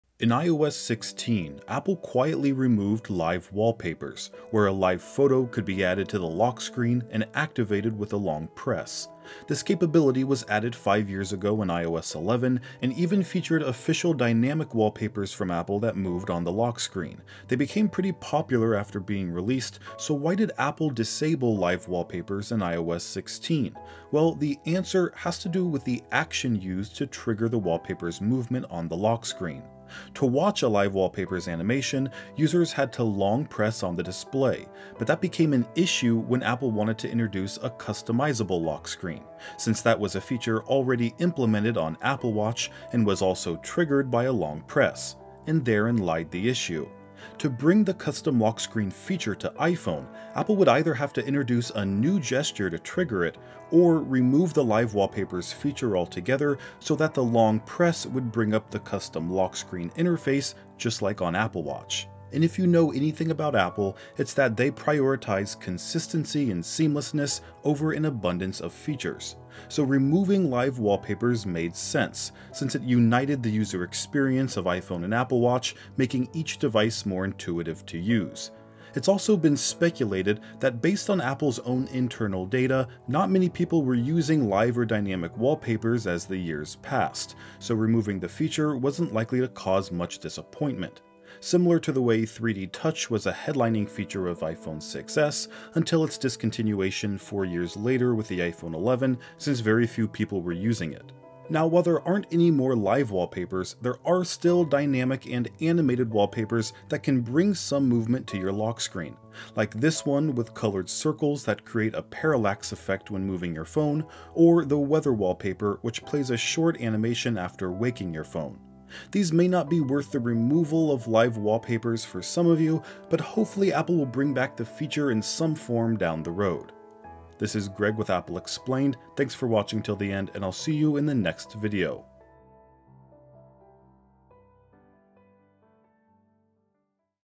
Why Apple Removed Live Wallpapers
In iOS 16, Apple quietly removed live wallpapers. Where a live photo could be added to the lock screen and activated with a long press. This capability was added five years ago in iOS 11 and even featured official dynamic wallpapers from Apple that moved on the lock screen. They became pretty popular after being released, so why did Apple disable live wallpapers in iOS 16?